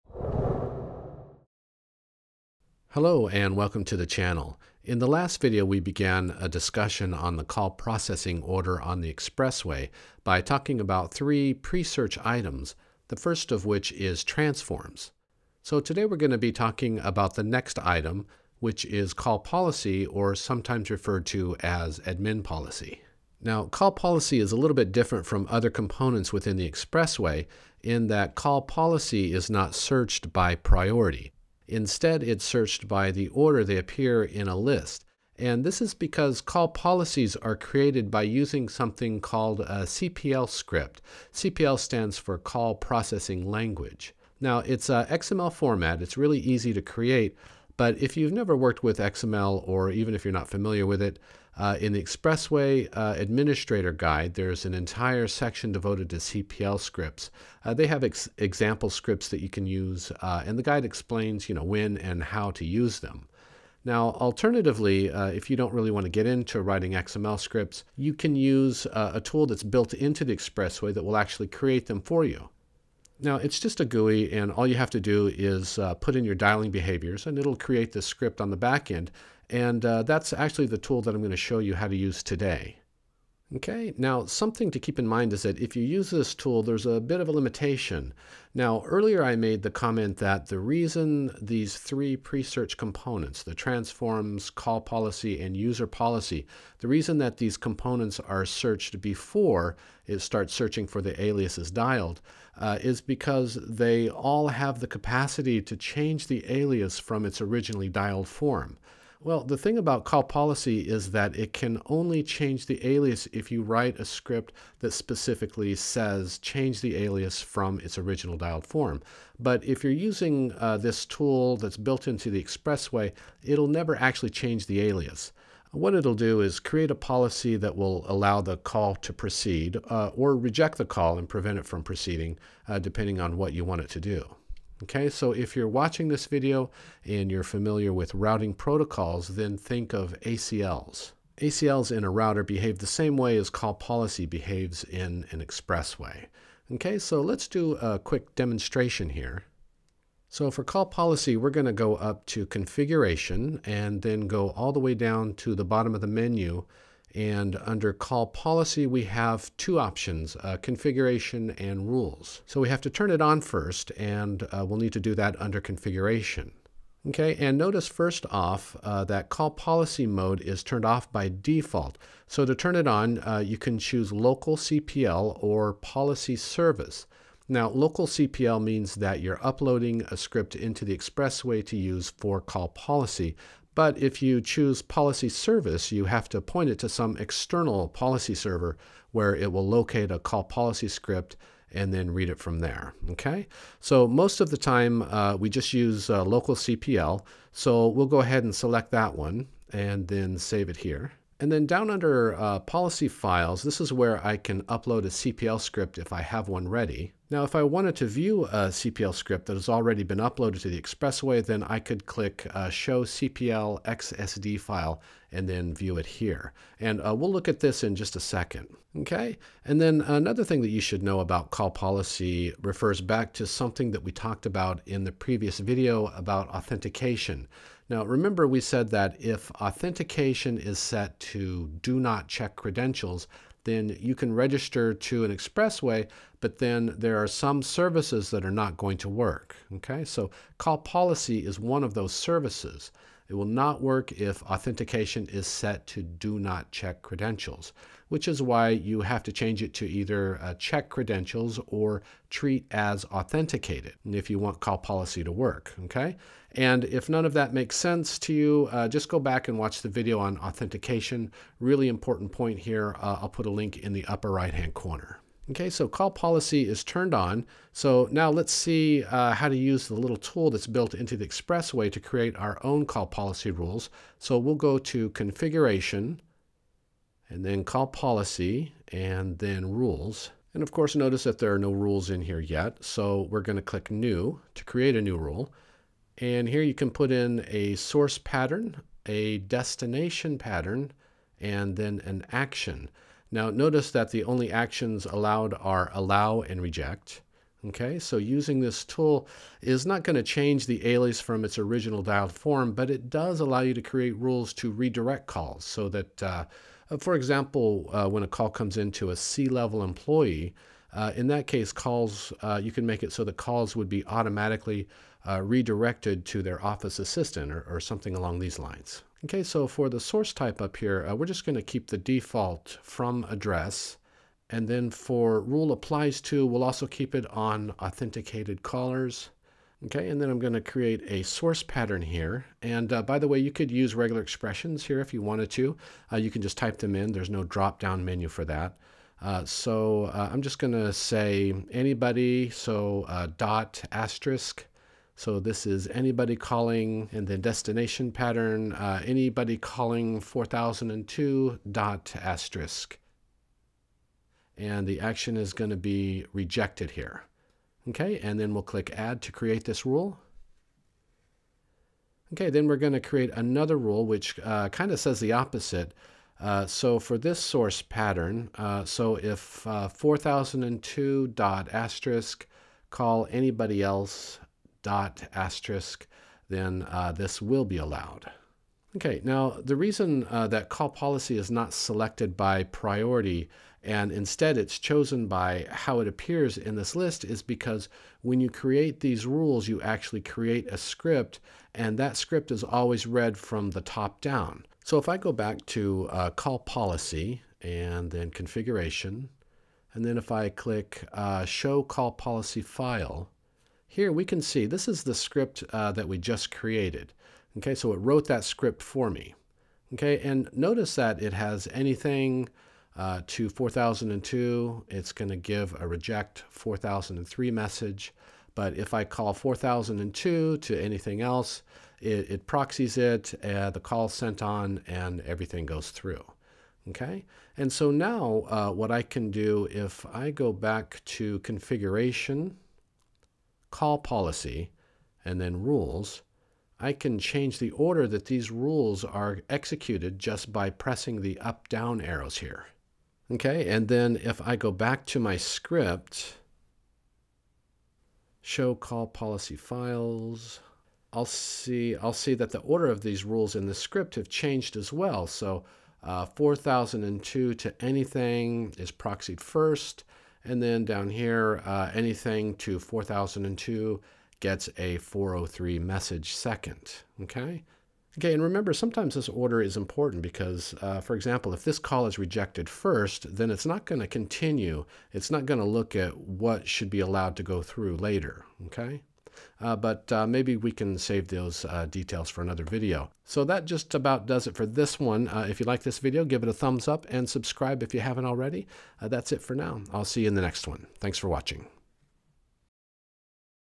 Call Processing Order - Call Policy - Expressway Series [9]
300-820 CLCEI
Call Policy, E154, SIP URI,
H323, SIP Endpoints

This video is about the second step in the Expressway's call processing order, which is Call Policy (aka "Admin Policy"). Call Policies are another way that the Expressway can transform an alias, such as an E164 alias into a SIP alias. Call Policies work by using CPL XML scripts. In this video, I show how to use a GUI on the Expressway to write a script for you.